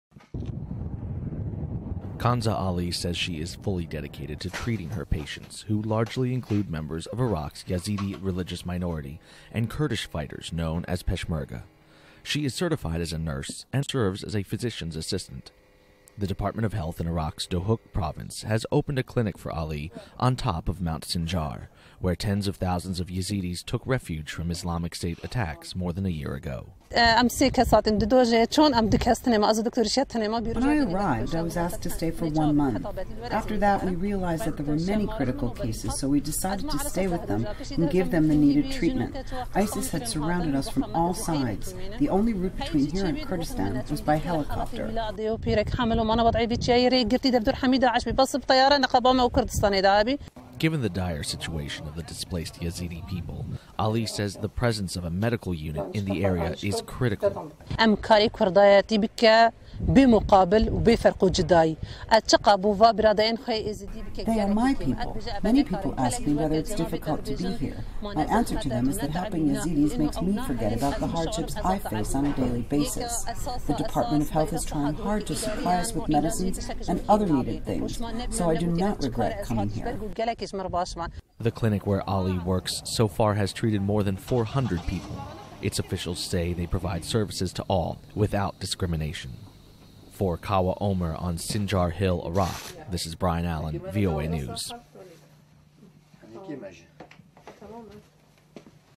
Syrian Nurse Aids Patients on Iraq’s Mount Sinjar
A female nurse from Syria is caring for the sick and wounded in Iraq’s Kurdish region despite a constant threat from the Islamic State group’s militant extremists. VOA’s Kawa Omer reports that Khansa Ali works in a clinic on Mount Sinjar, treating patients including Peshmerga fighters and members of the Yazidi religious minority group.